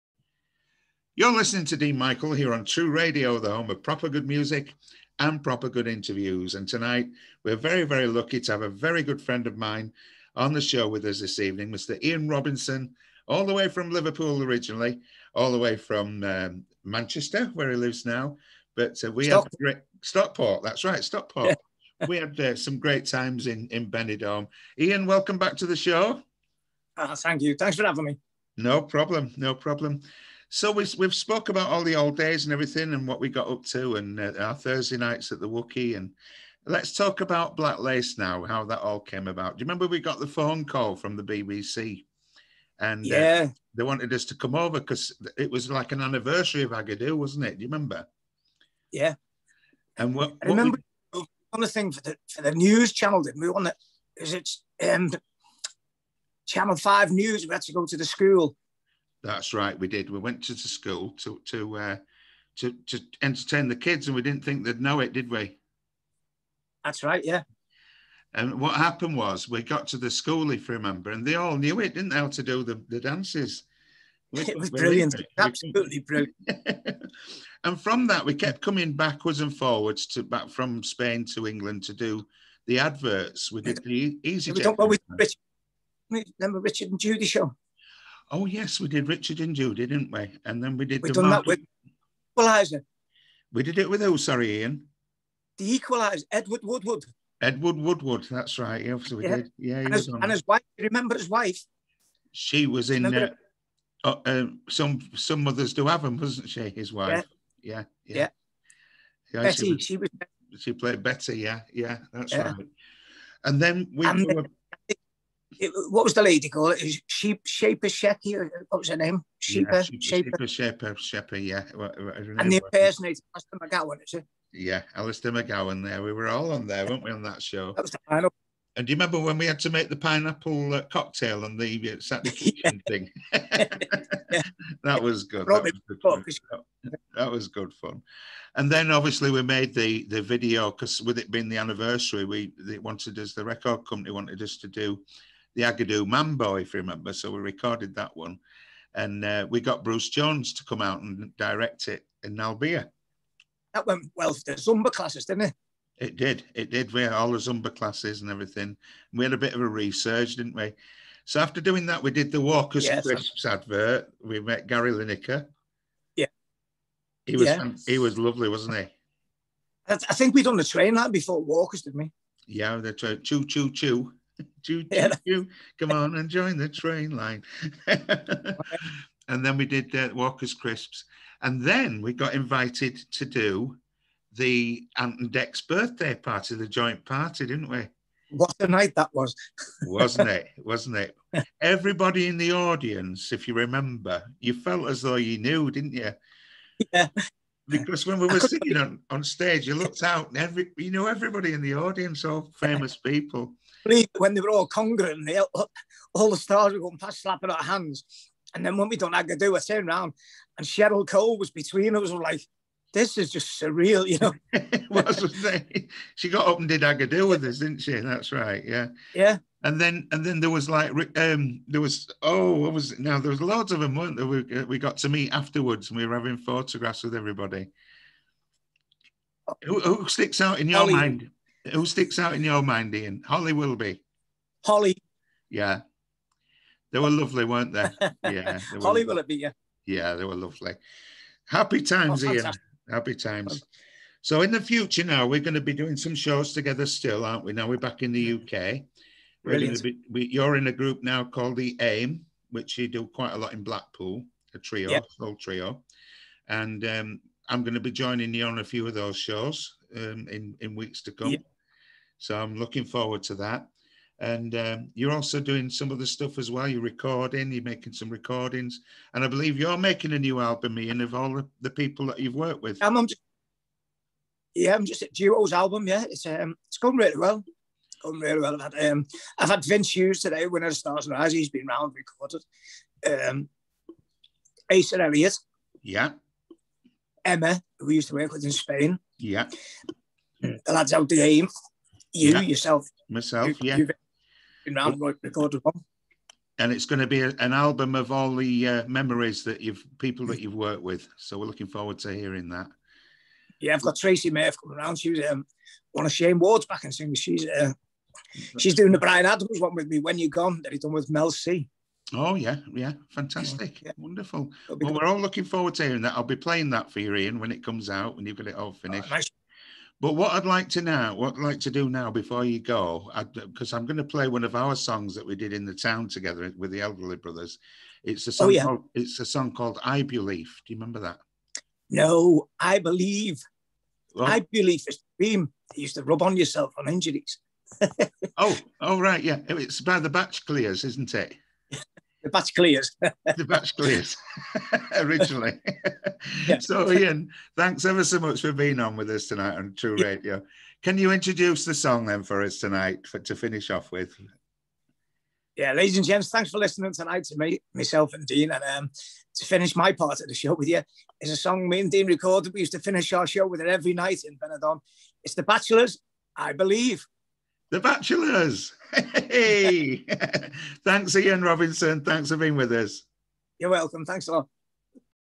Ian Roninson interview with Dene Michael on True radio part three
Ian Robinson in conversation with dene Michael part 3